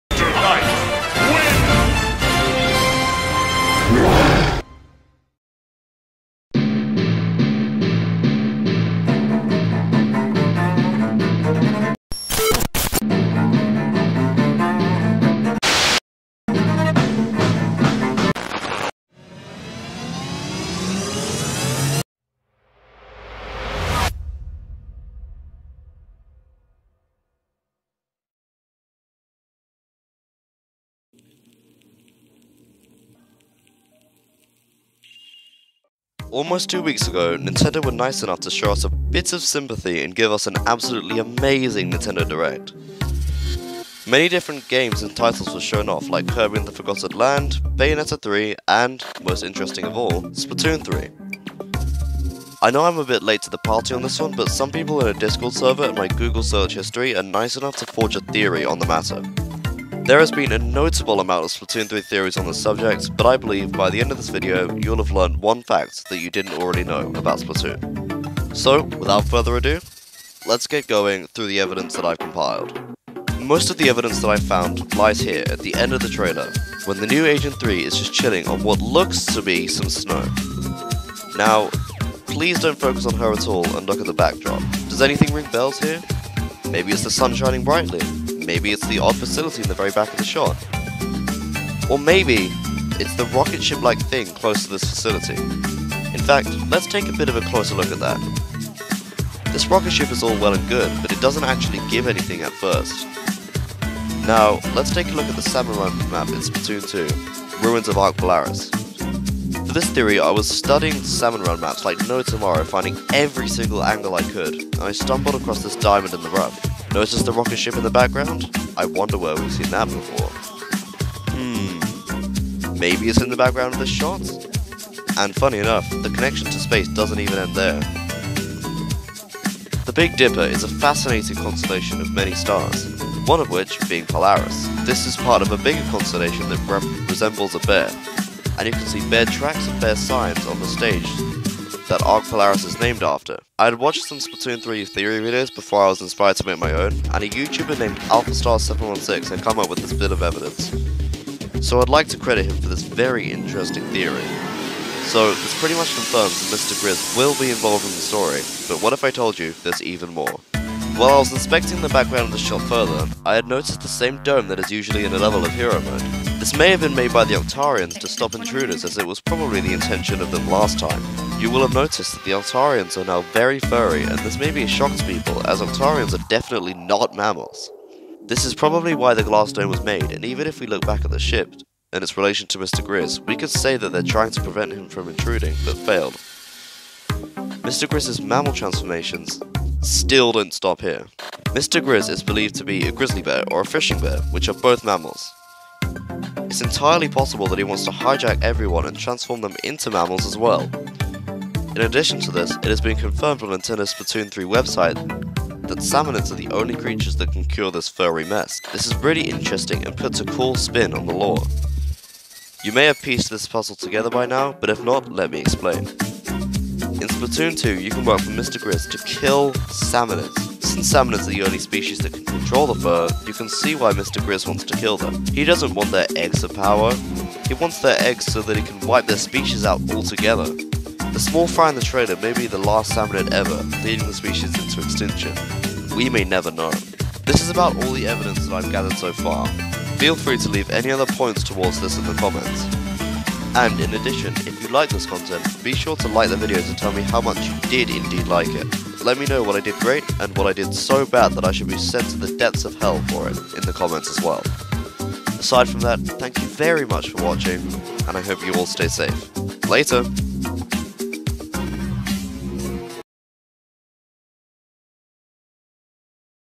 Why Mr. Grizz is the VILLAIN of Splatoon 3
I might be on to something here. My first theory, let alone Splatoon theory.